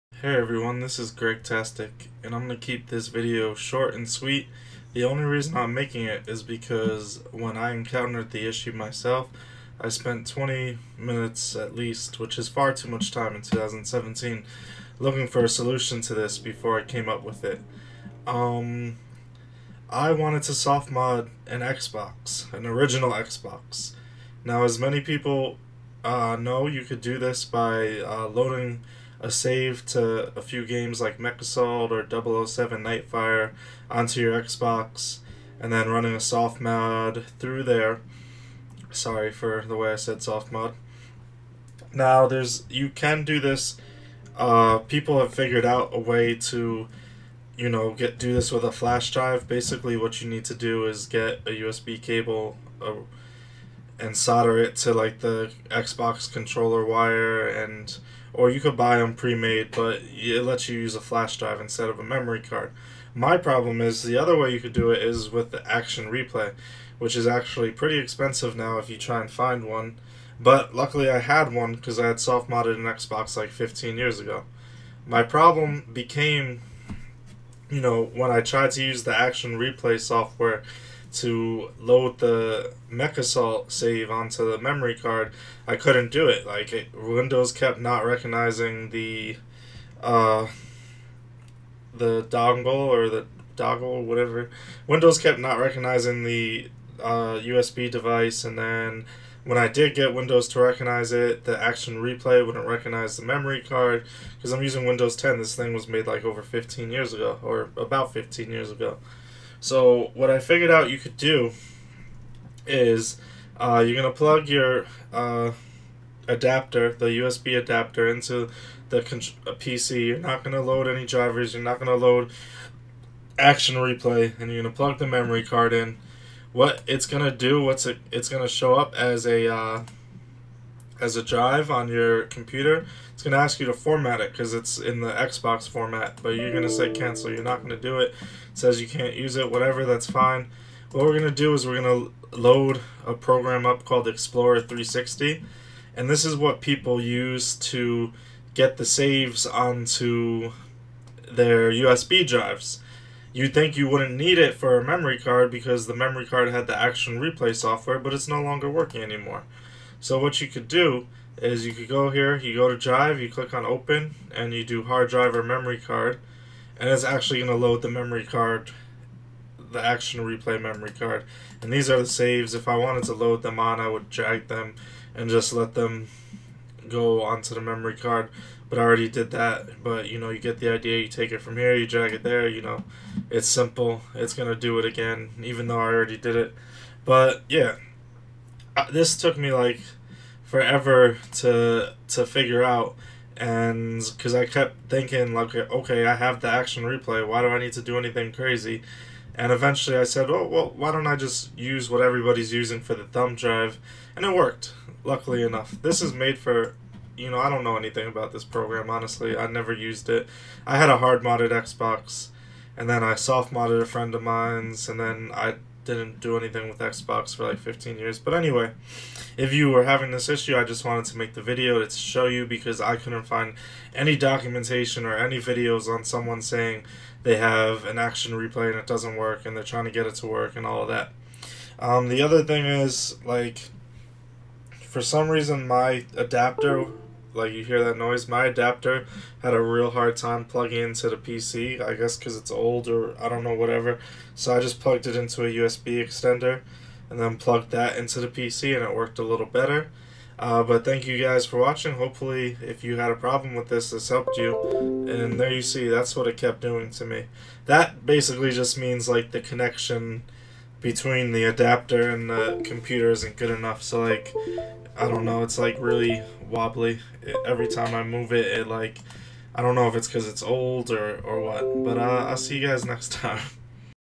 Xbox Action Replay Memory Card Workaround for Device not Recognized or Software Not Working
Are you trying to use a 15 year old Action Replay memory card on a later Windows computer such as Vista, 7, 8 or 10?

I will help you get around the "The last USB device you connected to this computer malfunctioned, and Windows does not recognize it." error as well as how to get around the Action Replay software no longer working.